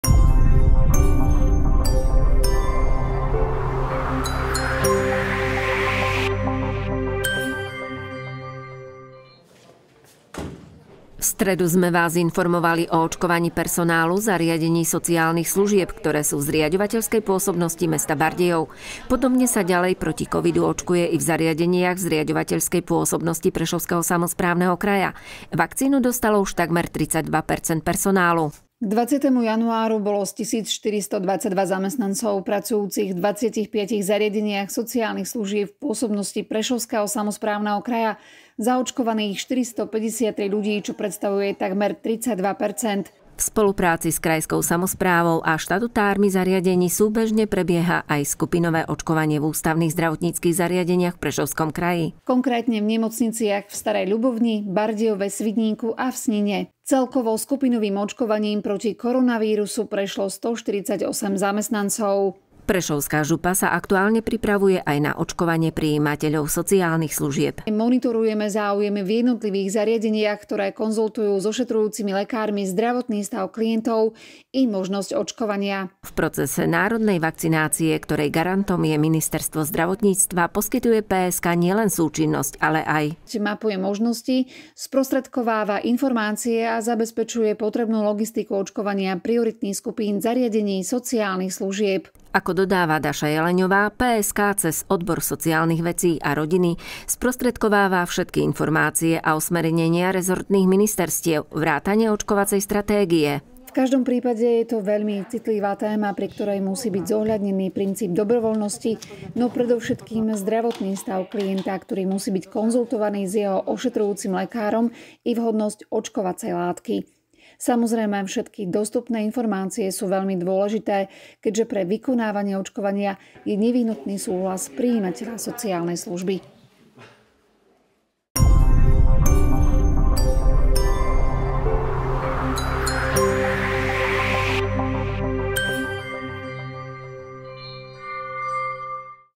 V kraji je zaočkovaných vyše 450 zamestancov sociálnych služieb
V PSK je zaočkovaných viac ako 450 zamestnancov sociálnych služieb
V stredu sme vás informovali o očkovaní personálu zariadení sociálnych služieb, ktoré sú v zriaďovateľskej pôsobnosti mesta Bardejov. Podobne sa naďalej proti COVID-19 očkuje i v zariadeniach v zriaďovateľskej pôsobnosti Prešovského samosprávneho kraja. Vakcínu dostalo už takmer 32 percent personálu.